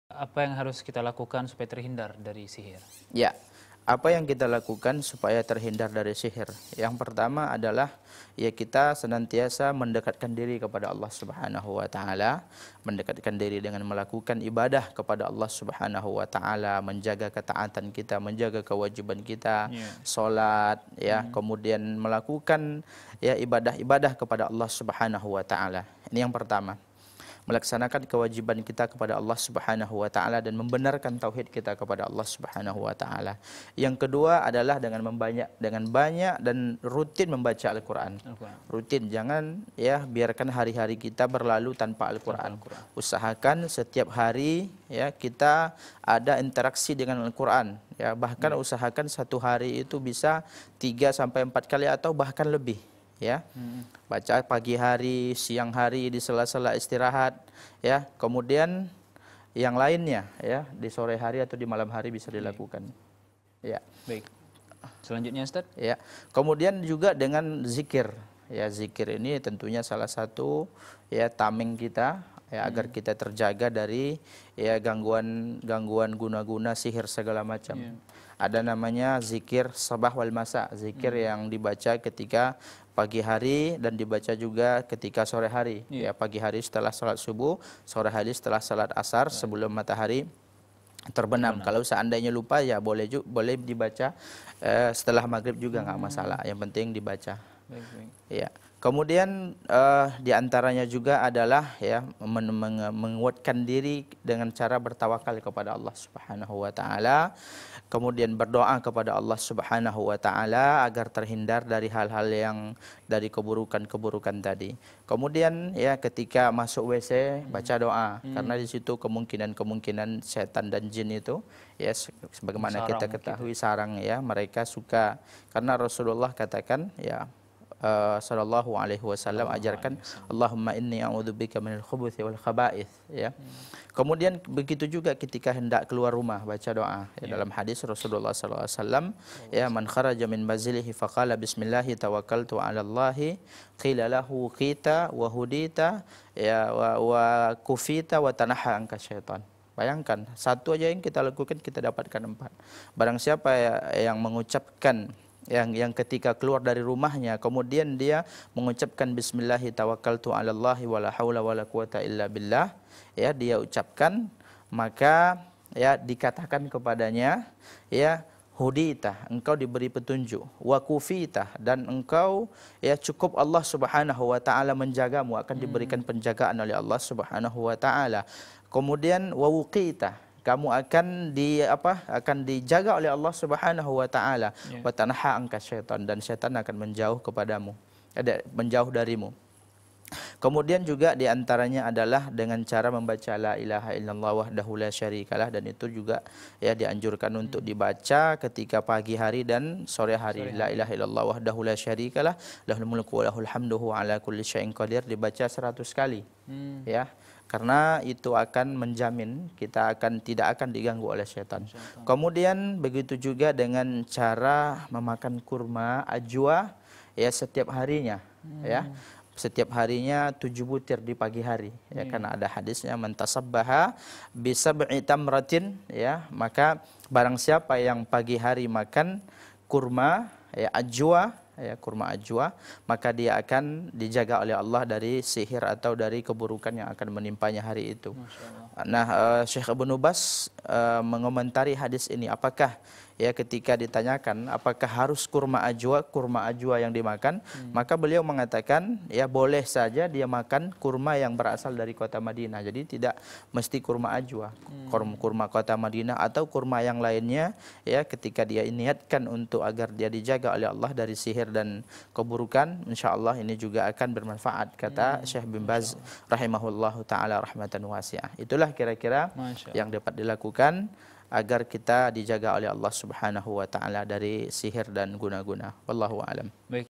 Kiat kiat Agar Terhindar Dari Sihir - Ustadz Ali Masnur, Lc. - Tanya Ustadz
Sahabat Keluarga Muslim, Apa pendapat anda terhadap Salam TV ?...
tulis di komentar dengan cara (nama-Alamat-Pendapat anda)